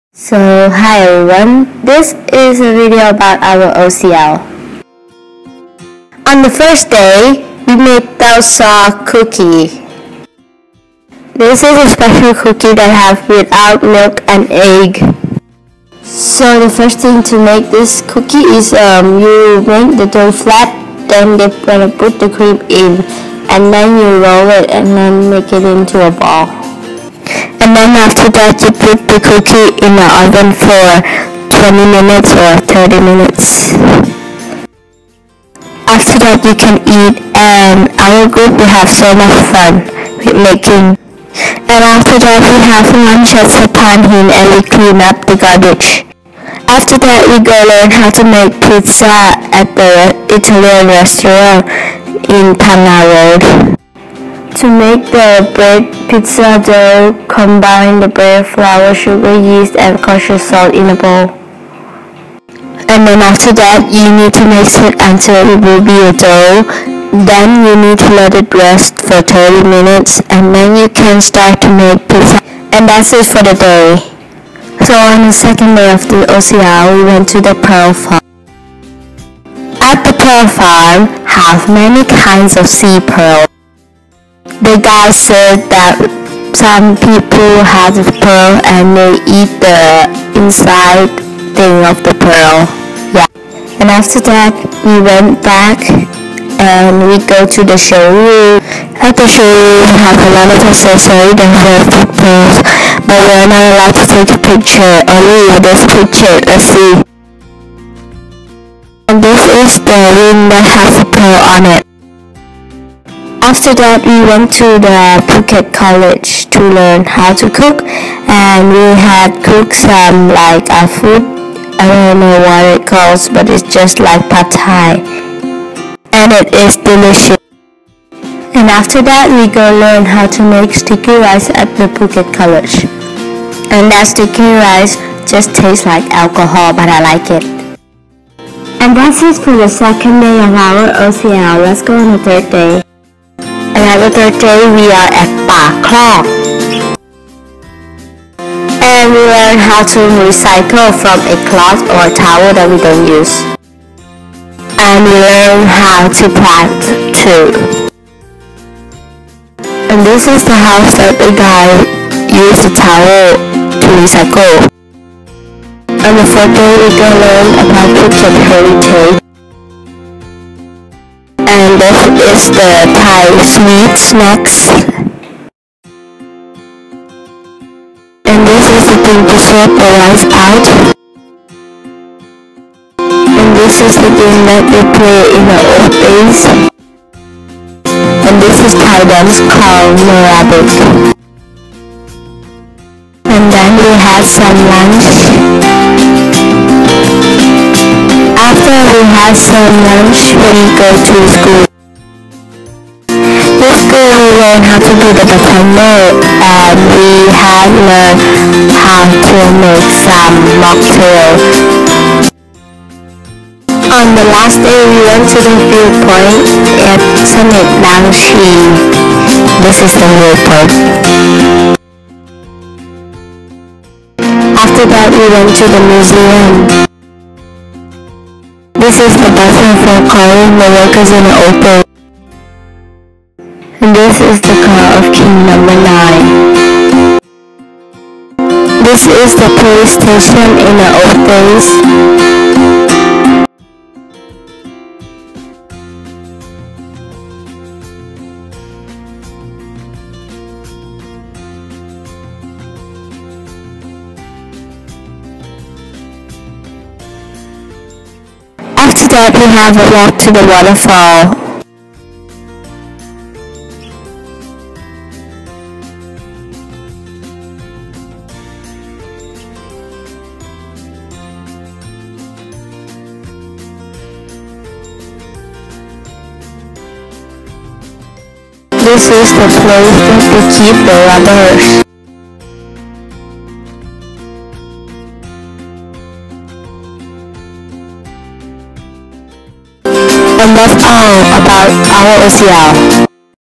Thailand Travel - Phuket-Phangga Vlog
A Drama Through Mass Media class project by M3/11
Travel Vlog by Yannah of M3/11
Mass Media class by Aj. Roma Martin
Featuring the Outside Classroom Learning (OCL) activity
Satree Phuket School - International Program